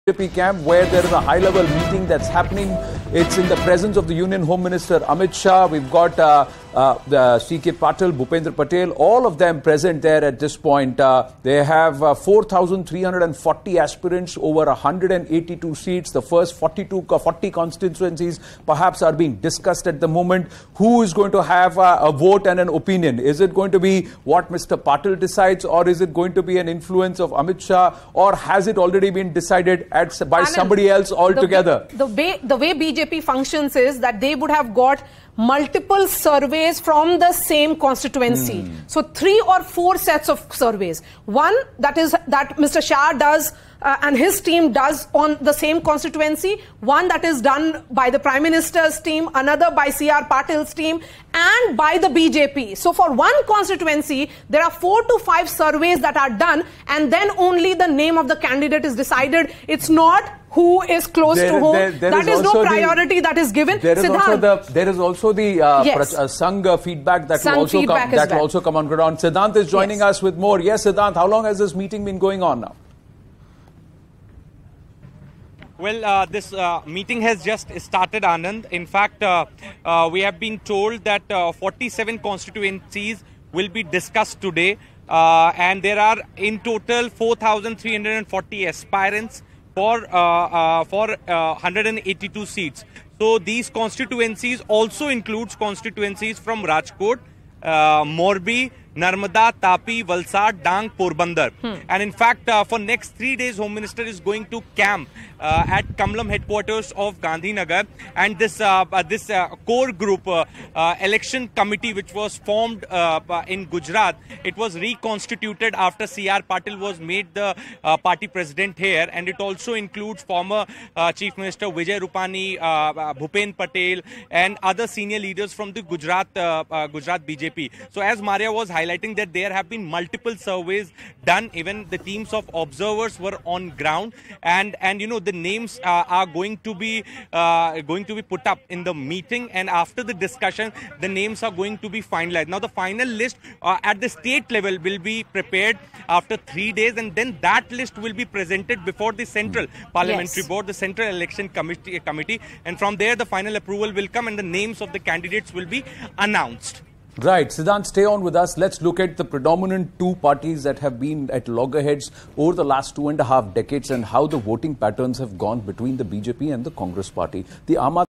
Home Minister Amit Shah Chairs Key BJP Meet Over Elections 2022 | Gujarat Elections 2022 | News18
4340 aspirants for 182 seats; 47 constituencies to be discussed

Reports from BJP office in Ahmedabad

Mega coverage broadcast with @AnchorAnand and @maryashakil